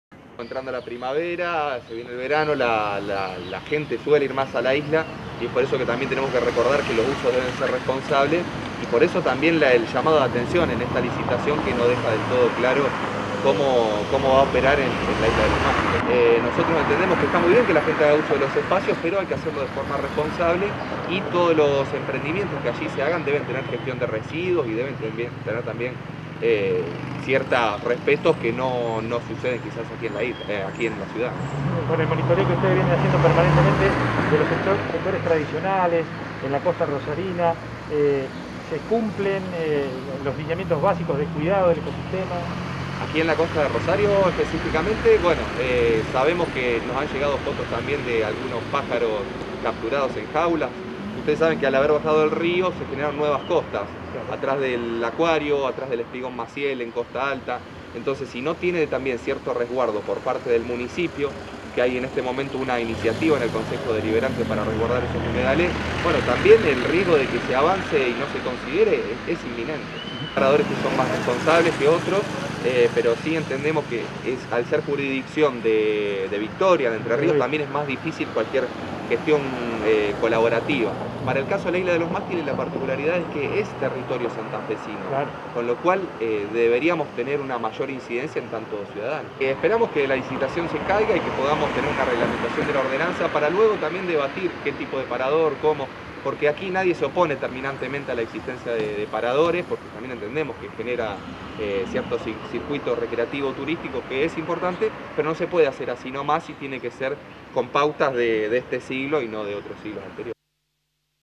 Alerta por la explotación en la isla Los Mástiles El proyecto no respetaría el cuidado del ambiente.
EL PROYECTO NO RESPETARÍA EL CUIDADO EL AMBIENTE
RODO MARTÍNEZ - MULTISECTORIAL POR LOS HUMEDALES

Alerta por la intención de explotar “islas de los mástiles” frente a Granadero Baigorria. Hay un proyecto que no contemplaría el cuidado del ambiente. En general, los paradores en zona de islas mantienen normas de convivencia con el entorno, aunque preocupa la afluencia masiva en el verano.